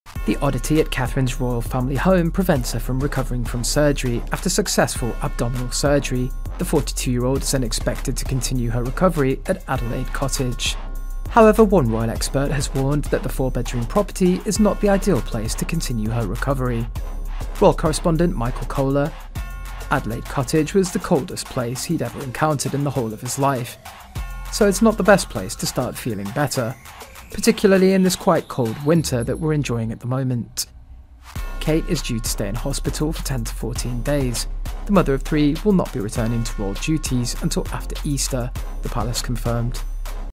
The Oddity At Catherine's Royal Family Home Prevents Her From Recovering From Surgery
After "successful abdominal surgery", The 42-year-old is then expected to continue her recovery at Adelaide Cottage.